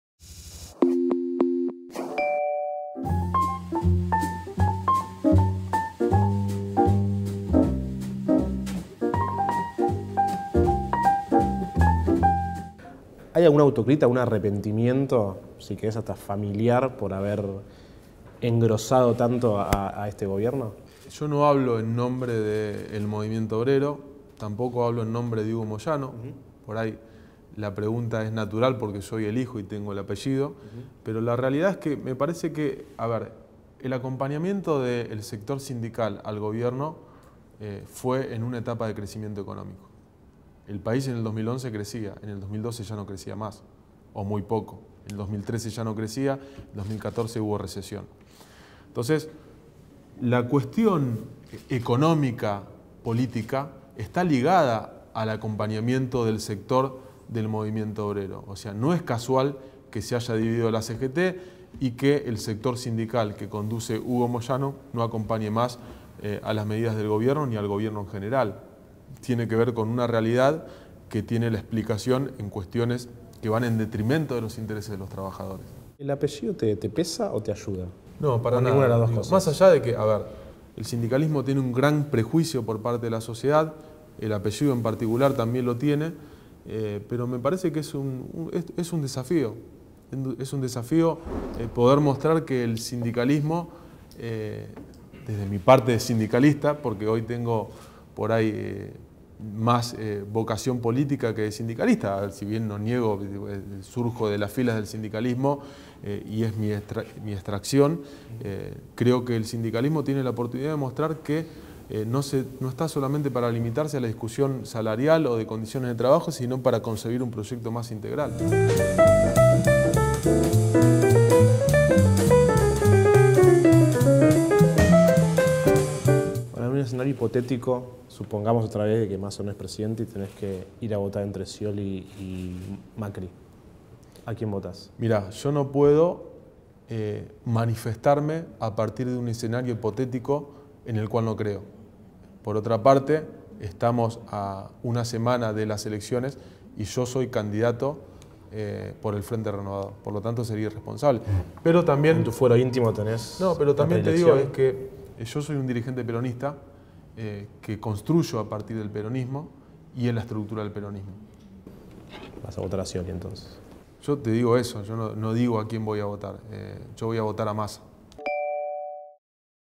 Café Político: Facundo Moyano dialogó con Infobae
Acusó a Macri de "ponerse un salvavidas de plomo" y criticó sus problemas para fiscalizar la elección. Reconoció que "salir terceros sería un mal resultado".